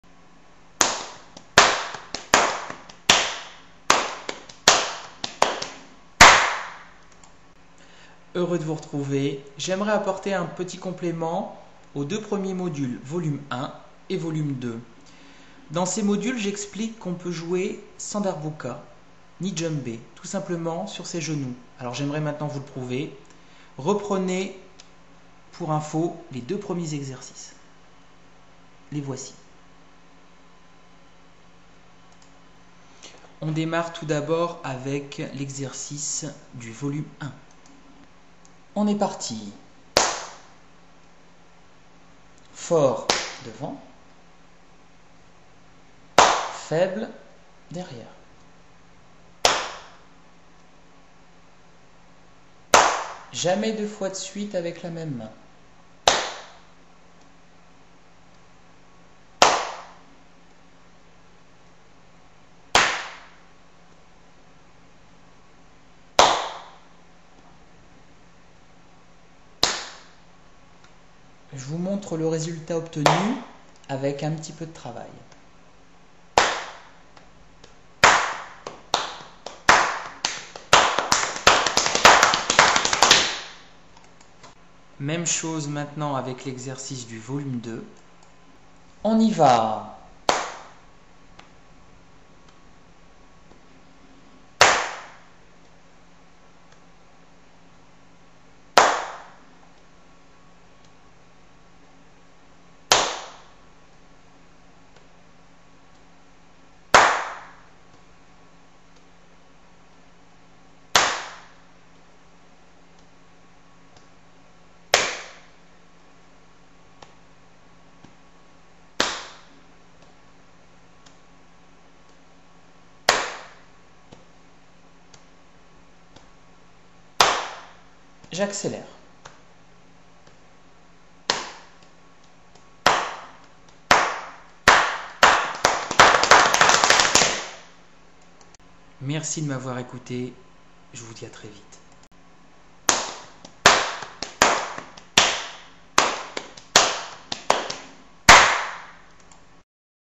COURS DE DJEMBE POUR DEBUTANT(ES) TOUT AGE (Volume 3)
Voici mon troisième cours de djembé gratuit ! J'en posterai d'autres... Et plus ces cours auront de succès, plus j'aurai envie d'en poster plein d'autres... Bien à vous. Carlos